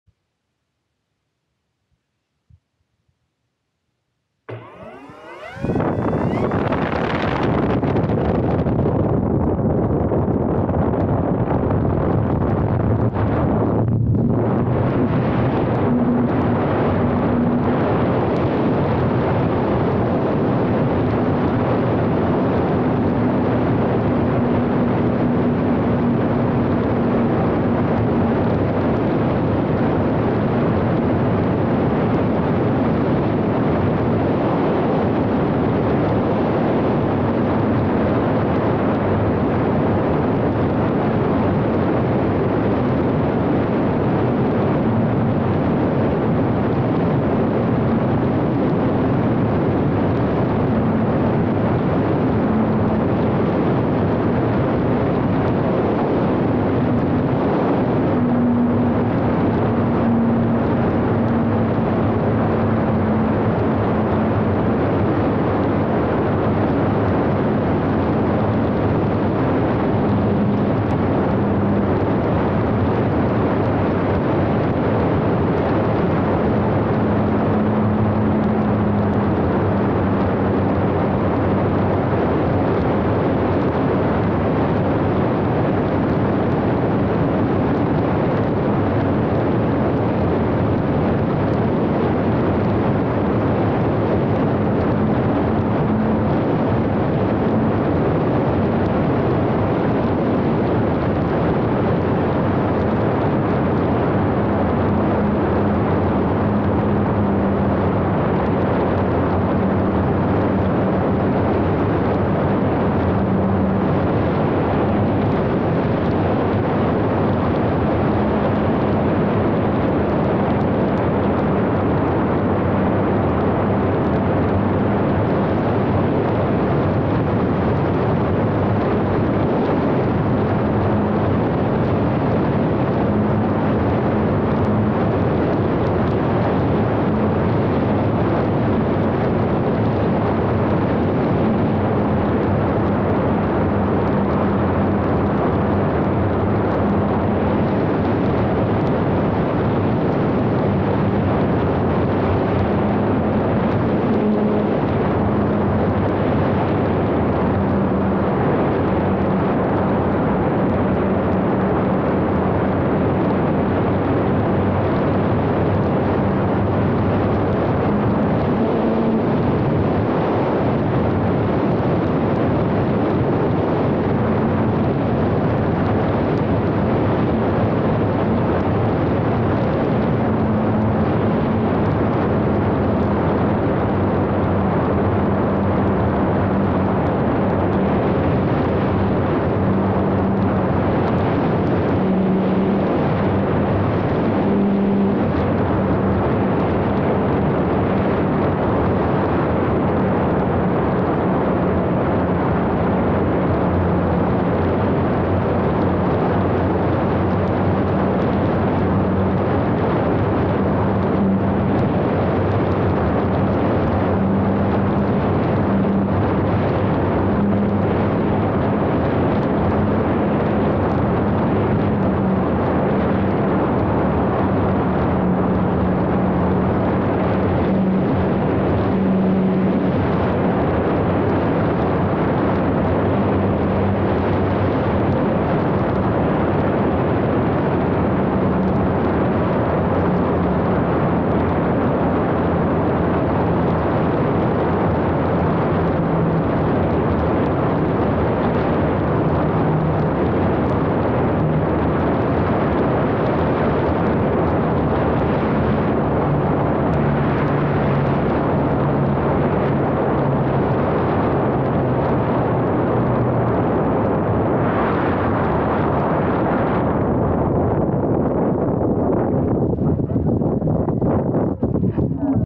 Lucky Lucy the Lancaster flight #3.
Beautiful morning to dust off Sam's  big Lanc and get her flown.